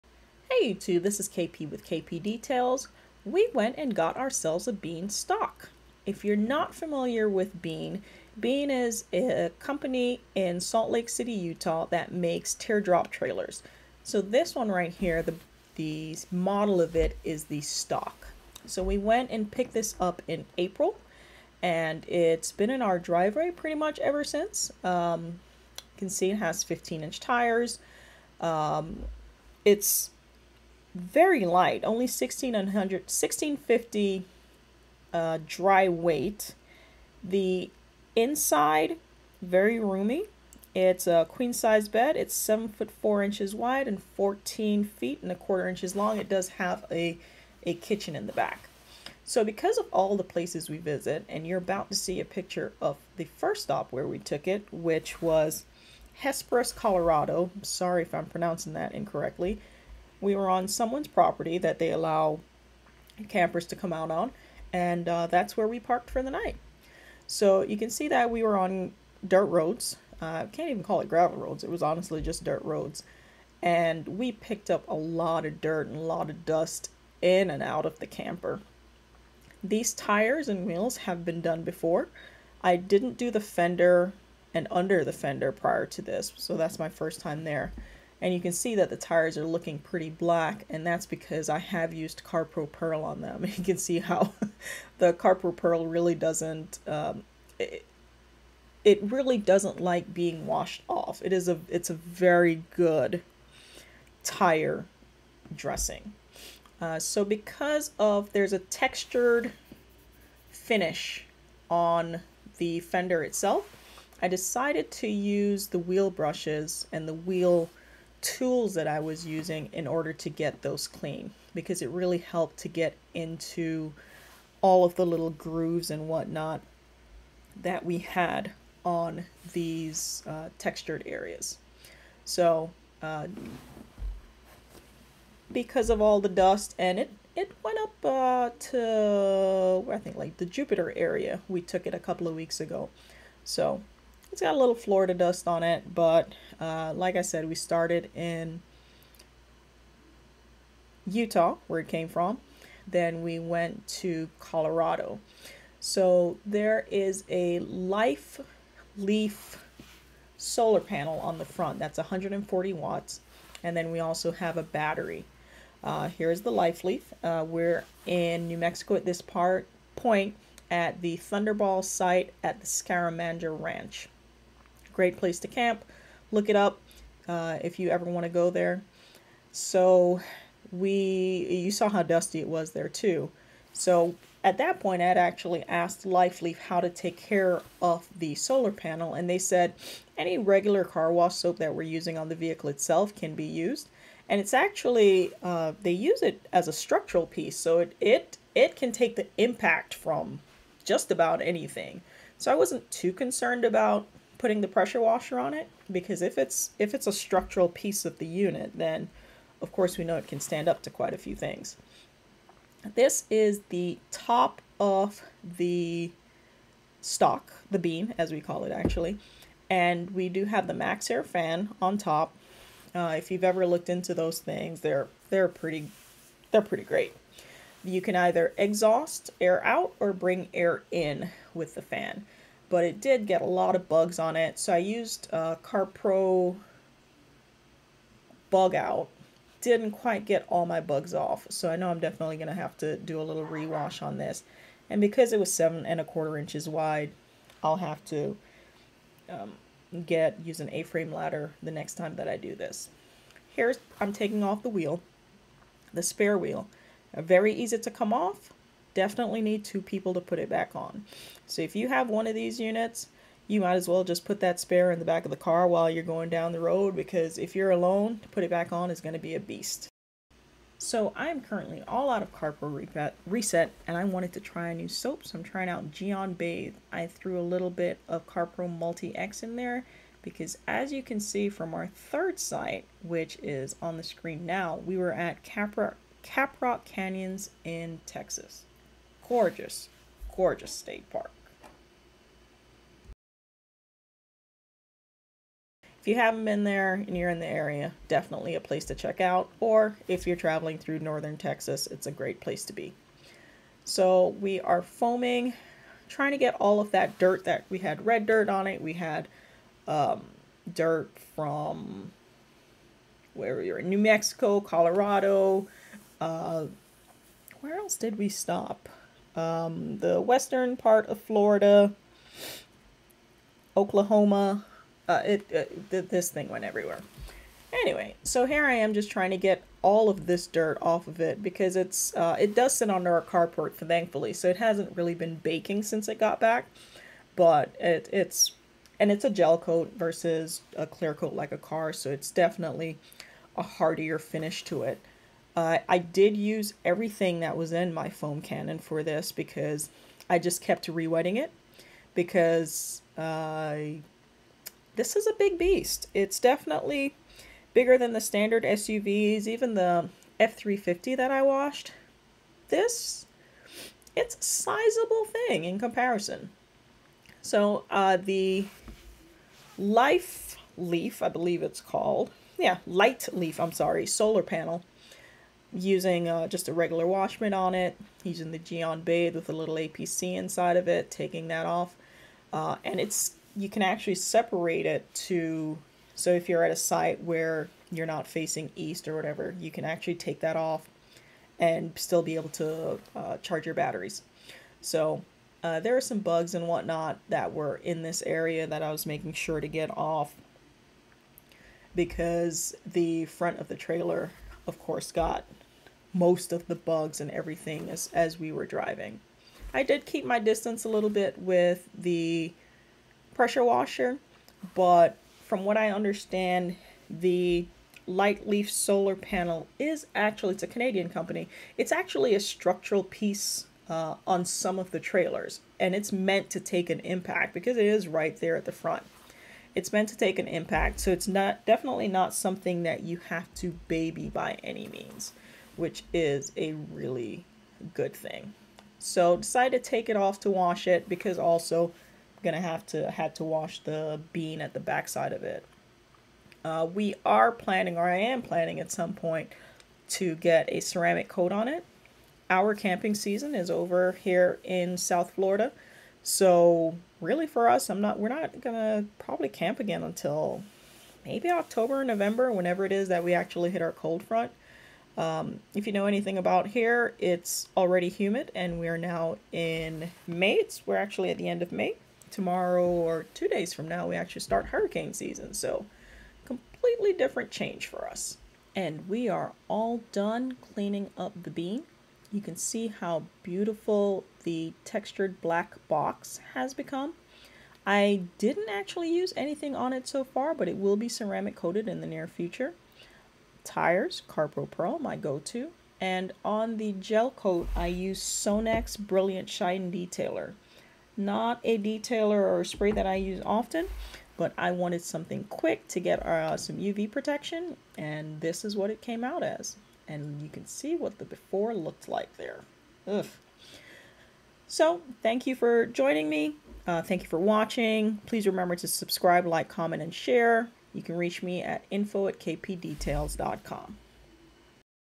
We bought a Bean! Here is The Bean's First Wash
We recently acquired a Bean Stock Teardrop Trailer, @BeanTrailer This is the first wash. The trailer was driven from the Bean factory in Salt Lake City, Utah to Fort Lauderdale, FL.

We traveled through Hesperus, CO to the Scaramanga Movie Ranch in Los Cerillos, NM and my last stop was Caprock Canyons, NM. The trailer camped in Checotah, OK, Cove Lake Recreation Area, Belden, MS, Montgomery AL and Old Town, FL before making it home.

Items used during this detail. Gyeon Bathe, CarPro Perl, CarPro MultiX, Adam's Tire & Rubber Cleaner and Sonax Brilliant Shine Detailer.